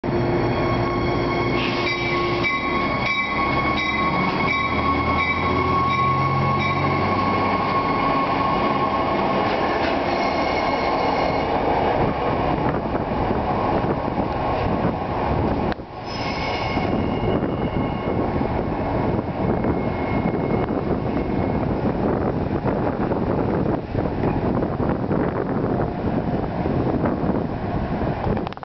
NJT River Line Train at Night
Departing Trenton Station at 8:15 p.m. at night heading to Hamilton Avenue, Cass Street, Bordentown, Roebling, Florence, Burlington, Beverly/Edgewater Park, Delanco, Riverside, Cinnaminson, Riverton, Palmyra, Pennsauken, 36th Street, Walter Rand Transportation Center, Cooper Street/Rutgers University, Aquarium & Entertainment Center in Camden. Unit's # is 3516.